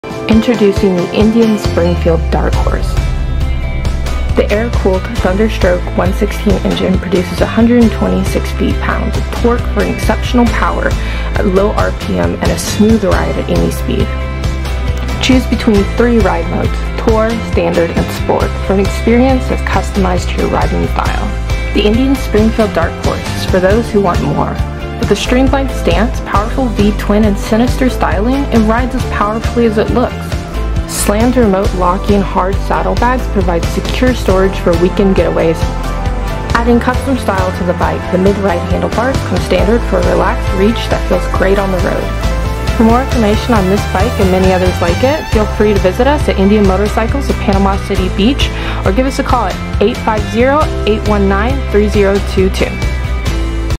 Indian Motorcycle Springfield Dark Horse
See what all the hype is about as Linda walks you through the new @Indian_Motorcycle Springfield Dark Horse. @indianofpcb Available Now.